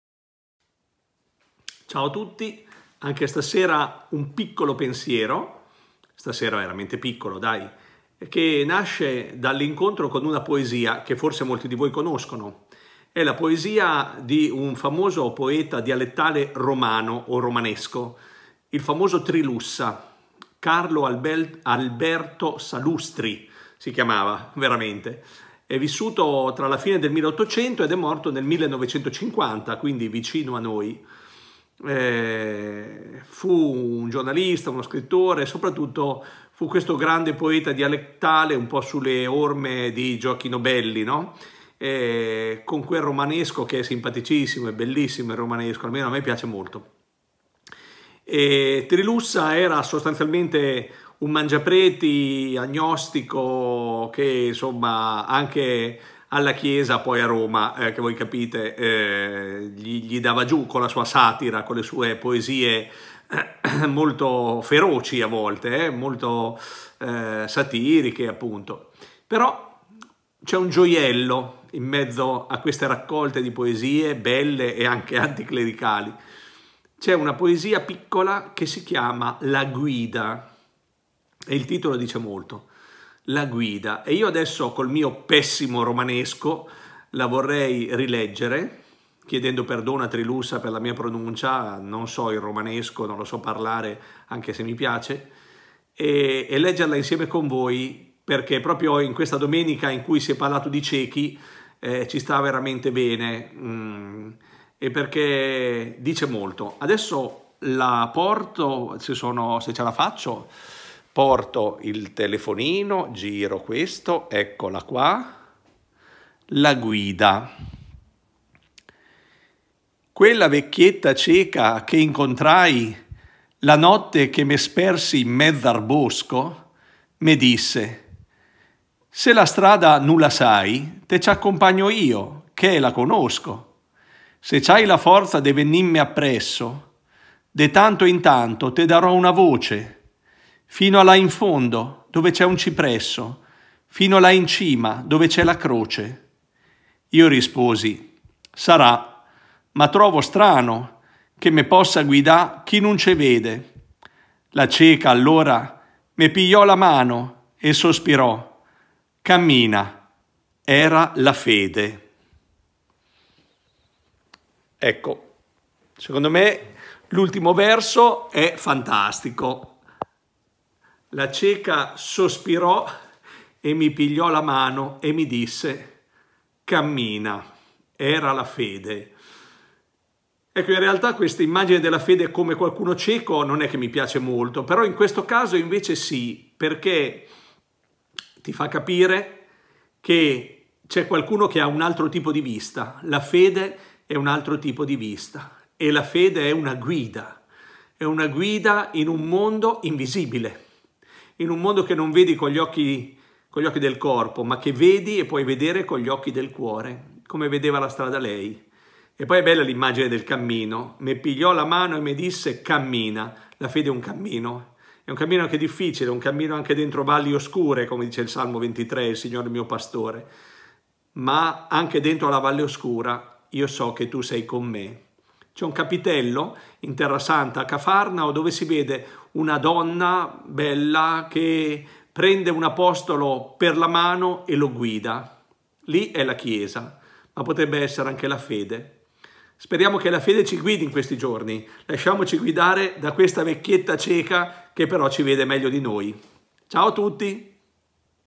La guida poesia di Trilussa
Piccolo commento a una grande poesia! C’è un errore: il capitello di cui vi parlo non è a Cafarnao bensì a Nazareth: scusate!